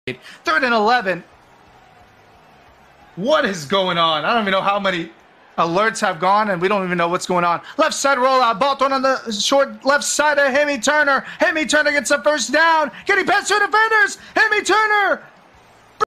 Taking Souls With These Ankle Breakers! | Legendary Bowl XIX | 49ers vs Falcons
Support the streamer! KiriCali on Twitch!